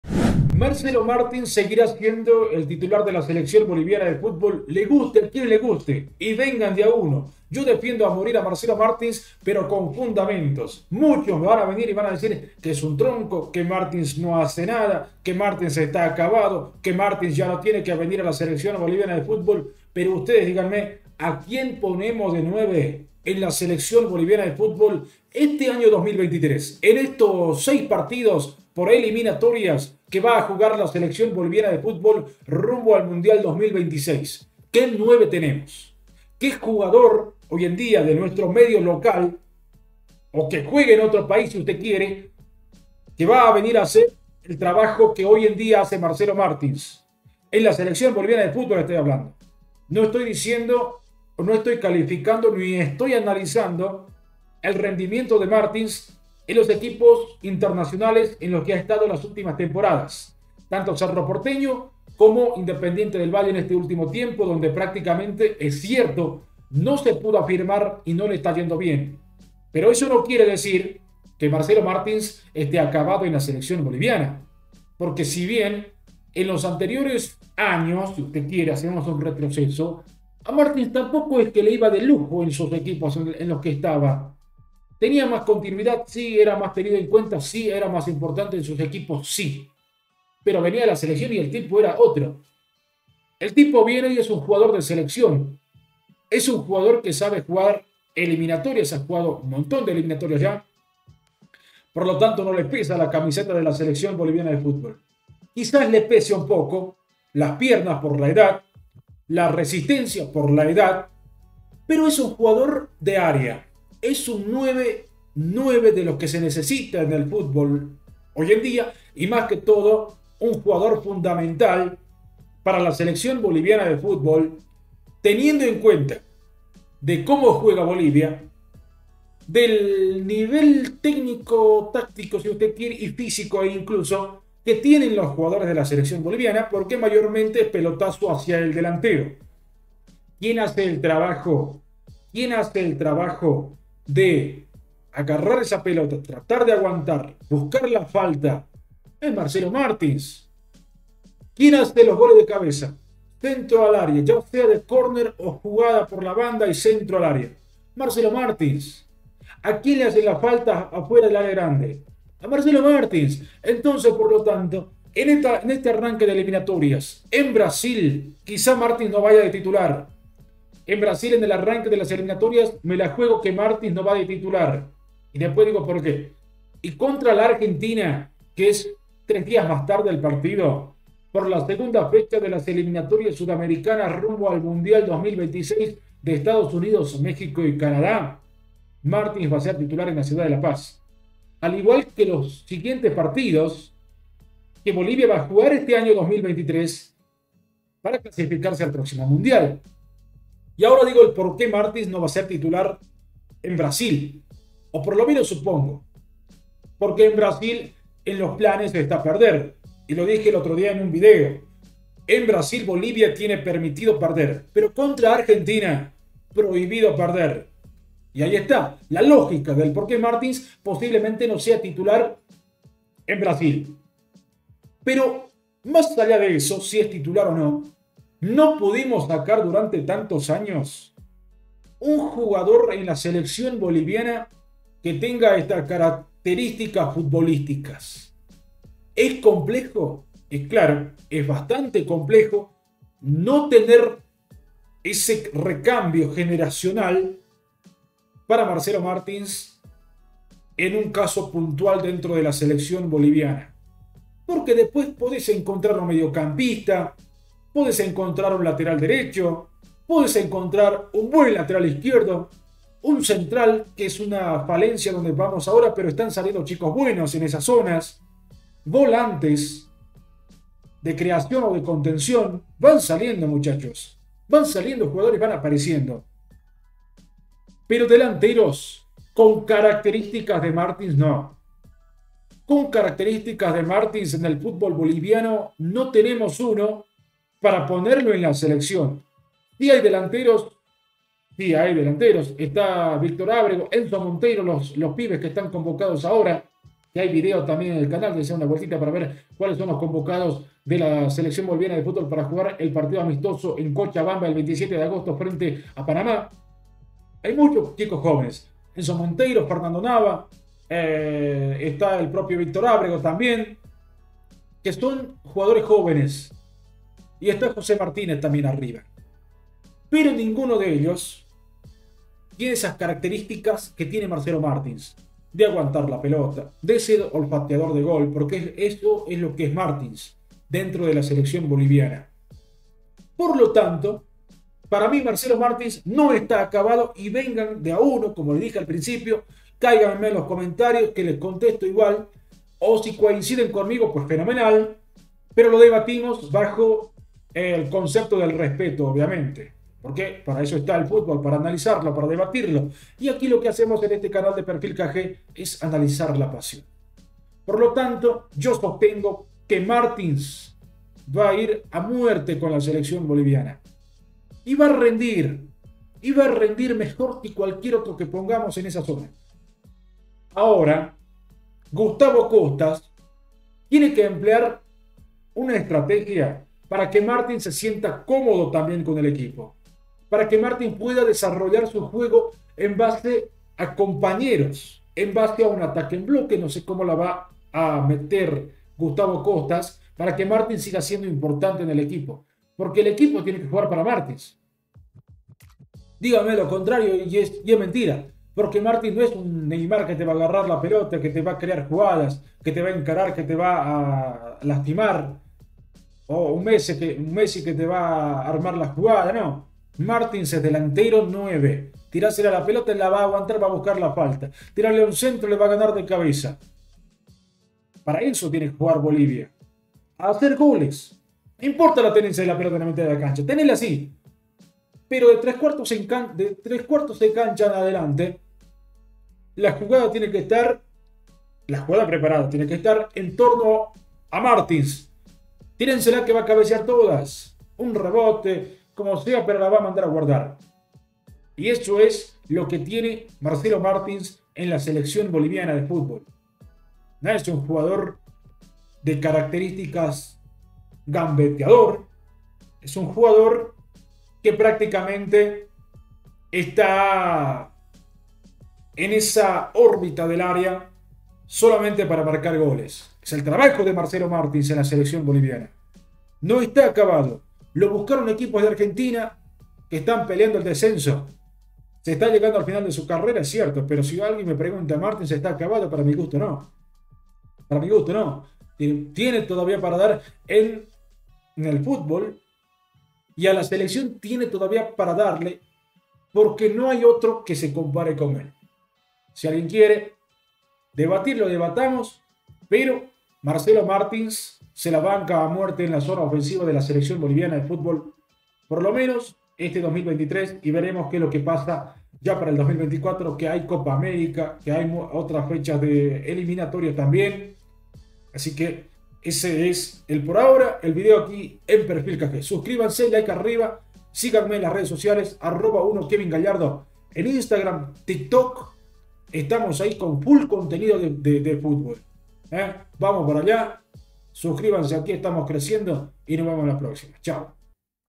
Martins SEGUIRÁ siendo el 9 de la Selección BOLIVIANA
En septiembre la Selección boliviana de fútbol arrancará un nuevo sueño mundialista para llegar a la próxima Copa del Mundo del 2026 en Estados Unidos, México y Canada, pero lo tendrá que hacer con Martins de 9 porque no hay otro jugador de igual características a él.

¿Coinciden?